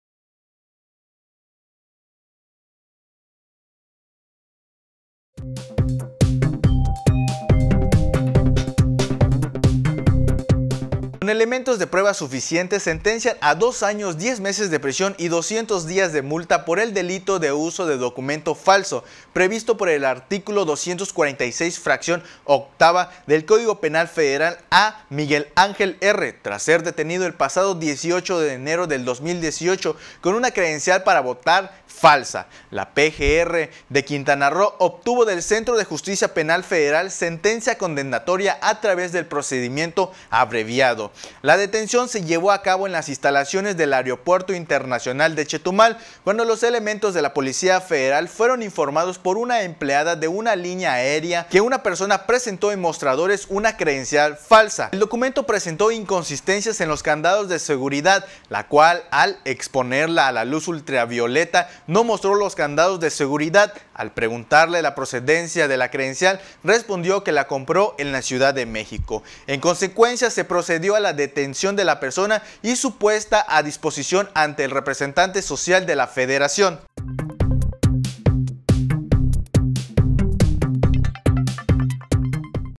SENTENCIAN A SUJETO A 34 MESES DE PRISIÓN POR TENER CREDENCIAL FALSA DE INE
Entérate de las noticias “EN CORTO”…SENTENCIAN A SUJETO A 34 MESES DE PRISIÓN POR TENER CREDENCIAL FALSA DE INE...